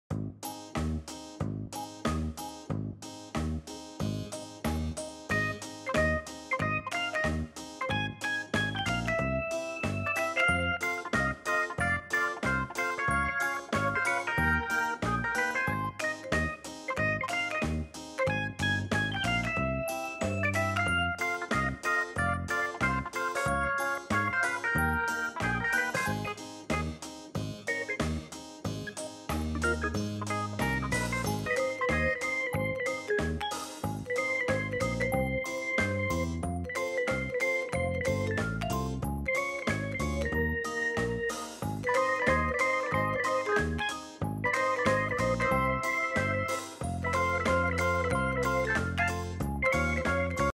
Walrus Jigsaw Puzzle Magic: Piecing Together Arctic Charm!
Dive into the world of Arctic wonders as we solve an enchanting walrus jigsaw puzzle! Join us in this mesmerizing journey of piece-by-piece discovery, as the puzzle reveals the charismatic charm of these magnificent marine mammals in their icy realm. Watch as we engage our puzzle-solving skills to assemble the walrus scene, capturing the essence of the Arctic's beauty. Are you ready to embrace the magic of puzzle-solving? Let's embark on this short adventure together!